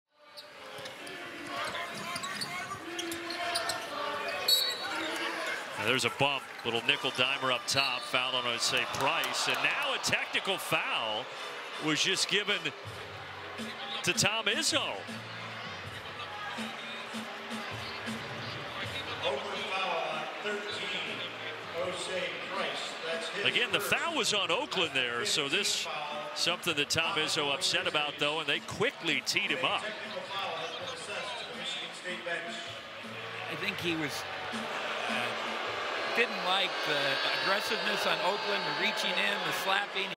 Tom Izzo gets called for technical foul while wearing elf sweater
During the college basketball game between Michigan State and Oakland, Michigan State coach Tom Izzo was called for a technical foul after yelling at the refs. The sequence made for a funny visual as coach Izzo was wearing an elf sweater.